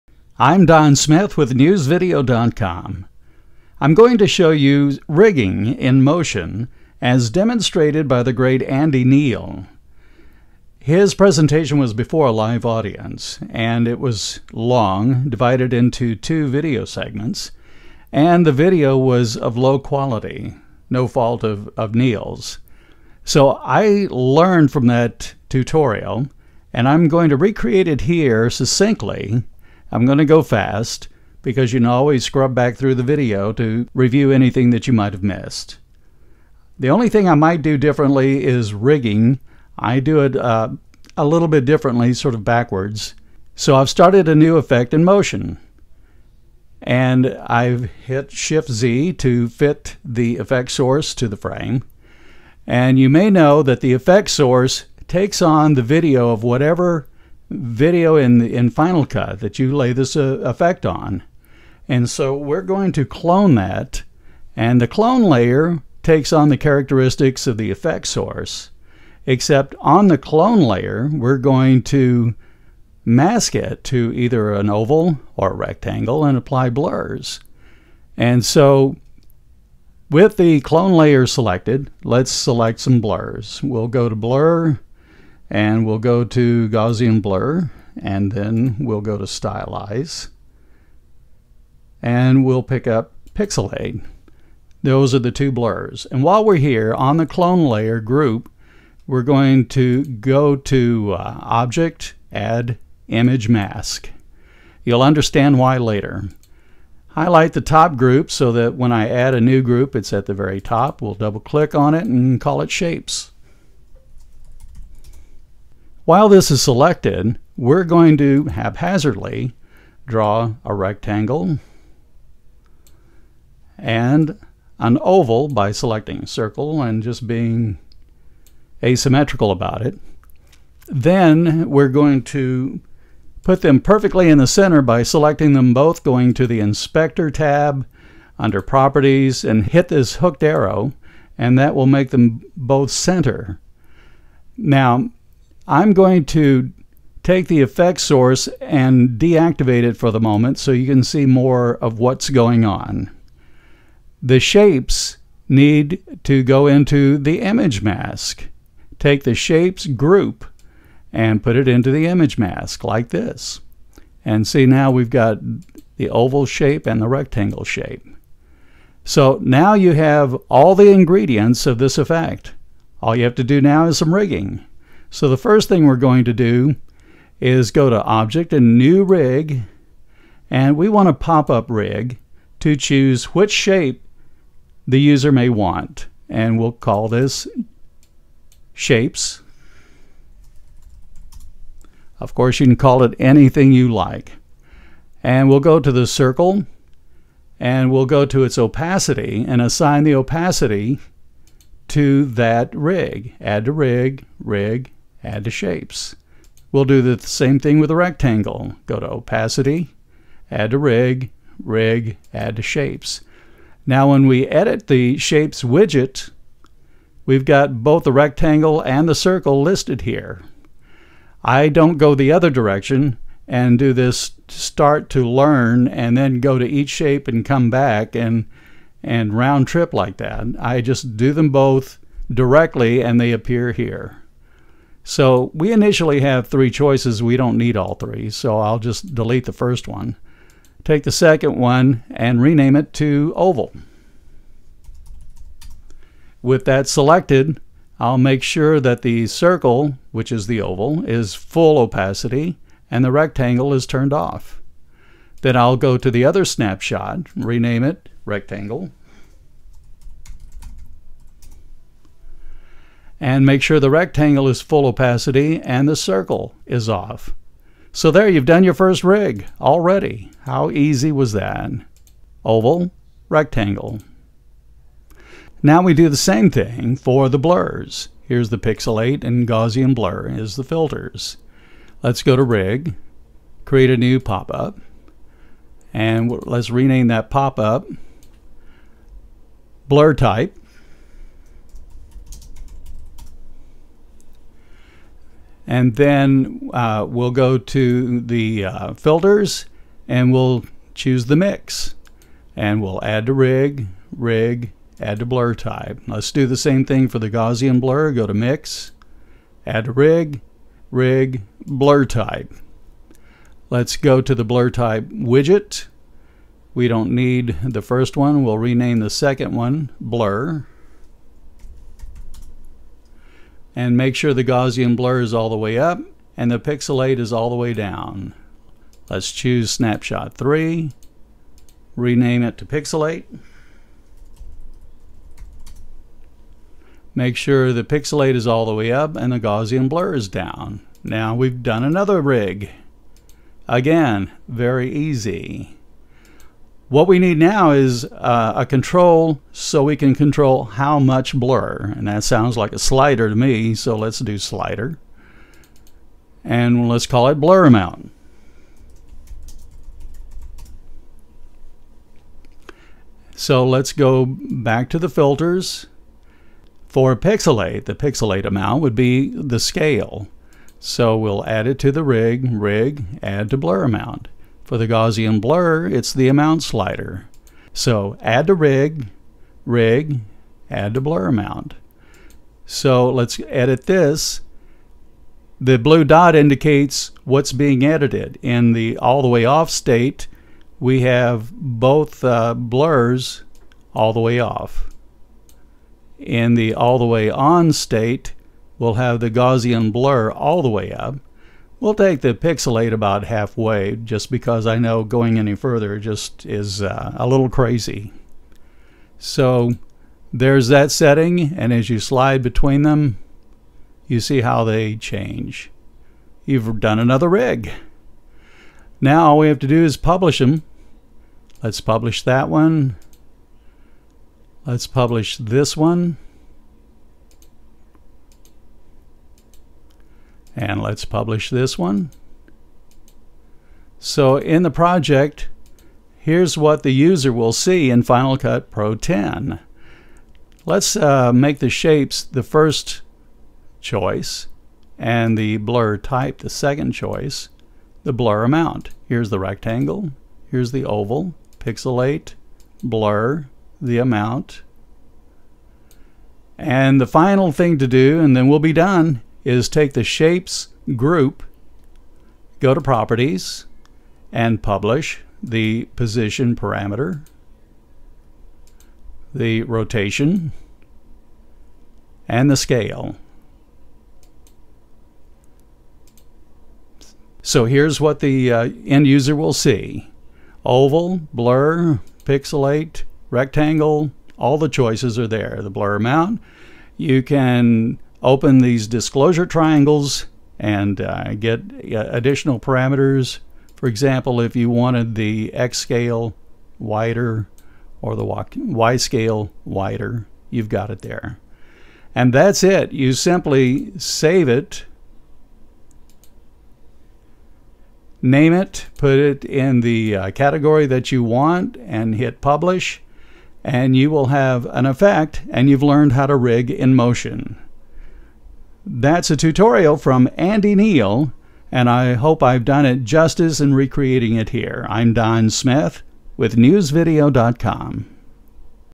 Motion Rigging Fast
This is a re-creation of an Andy Neil tutorial that he did before a live audience. That presentation was poorly captured and was extended over two video segments. However, it was the first tutorial on Rigging in Motion that I understood and I'm excited to recreate Andy's presentation here in a fast-moving presentation with high video quality.